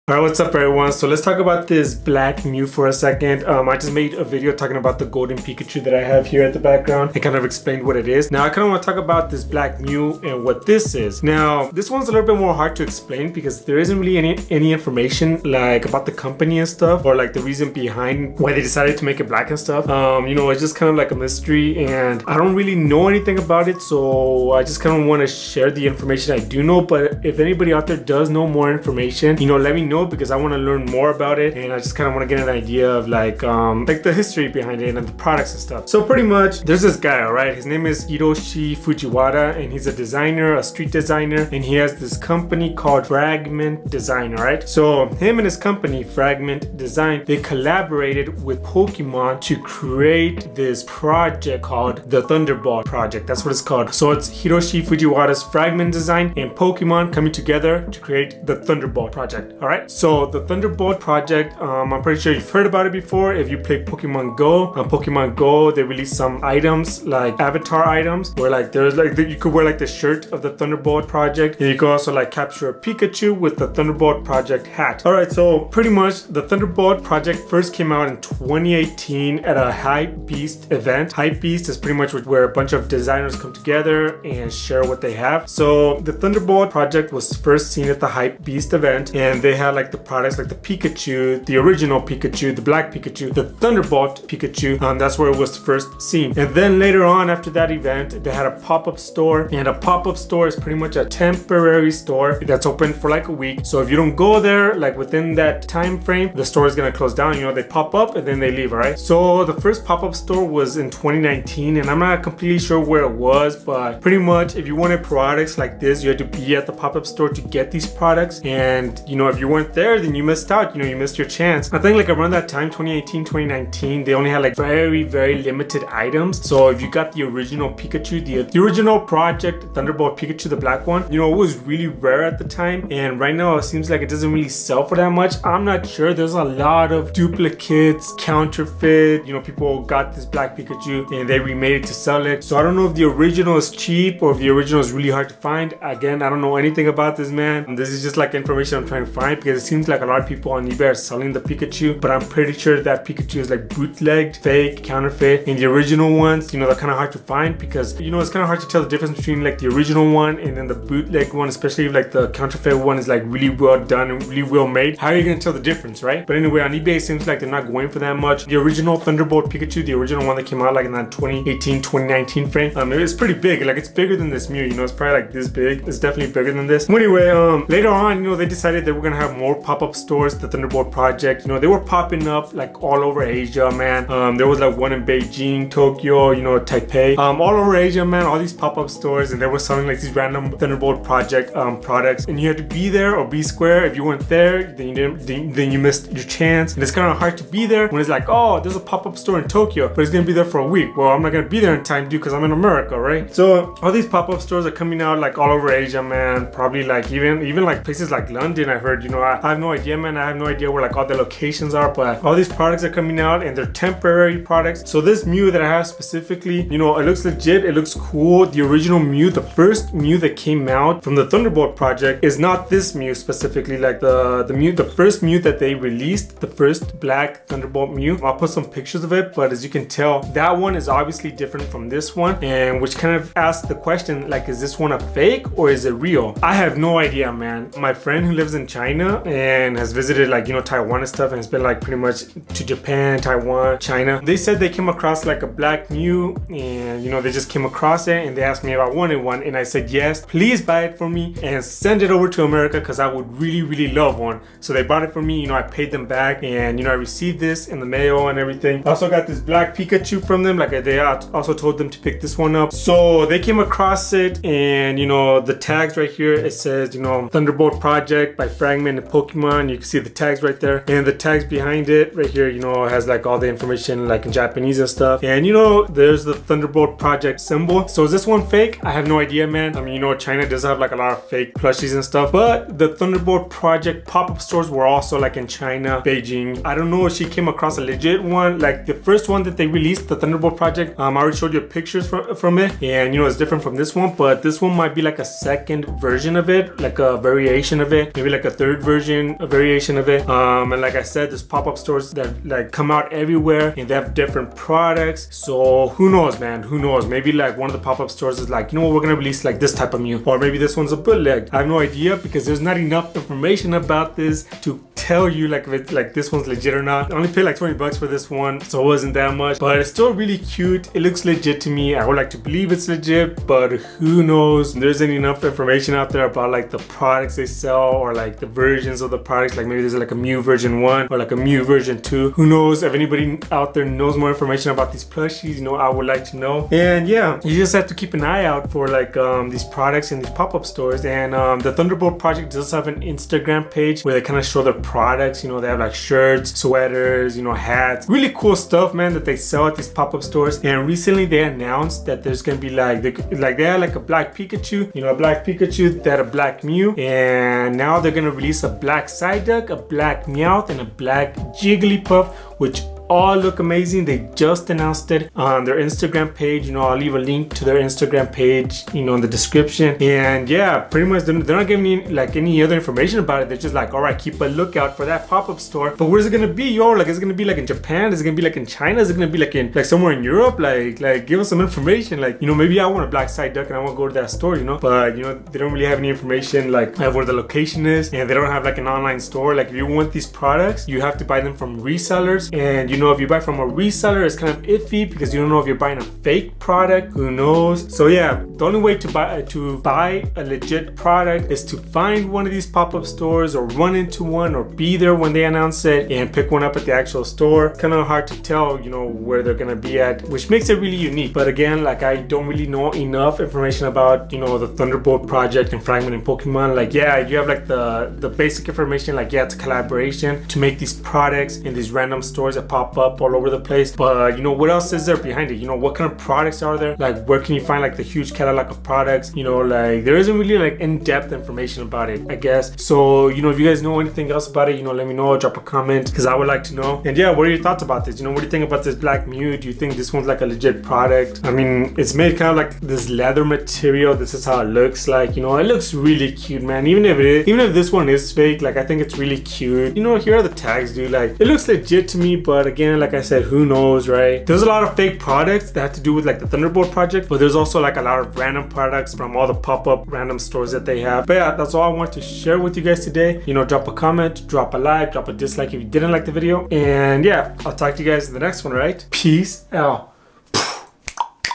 The Fragment BLACK Mew - What Is It? ポケモン
The story behind the Thunderbolt Project's Black Mew^^